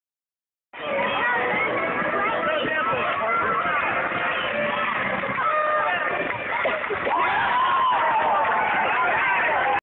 Caleb Easley scores just short of a NBA 3 pointer
Caleb Easley scores a 5th & 6th grader 3 pointer during the valley YMCA Championship game in the U.S. Airways Arena.